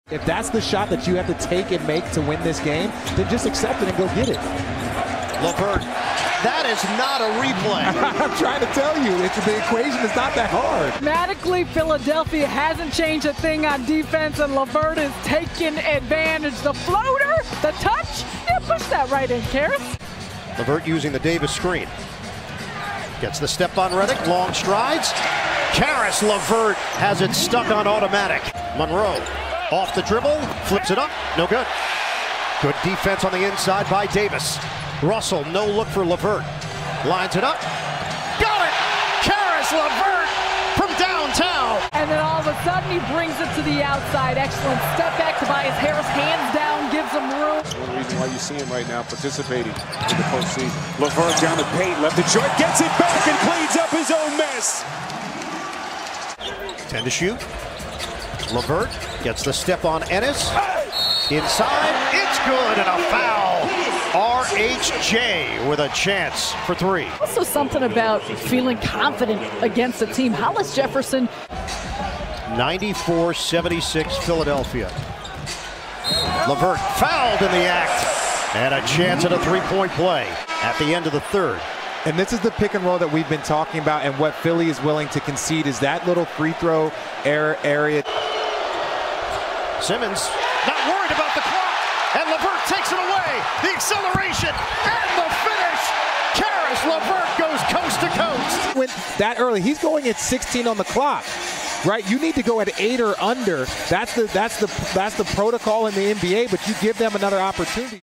12. Caris LeVert vs. Sixers | 2018-19 Top 15
As playoff basketball returned to Brooklyn, Caris went for 26 points against the Sixers in Game 3.